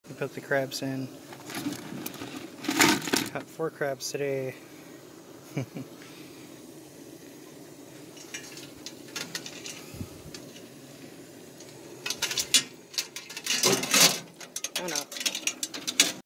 Cooking Crabs Goes Wrong
Cooking live crabs in boiling water is fast, have you ever tried steaming crabs before? It's hard, first you have to invite into your sauna for four then you have to hope that the first crab in isn't the first one out! I hope I'll have better luck next time to keep them in longer, they'll come out bright.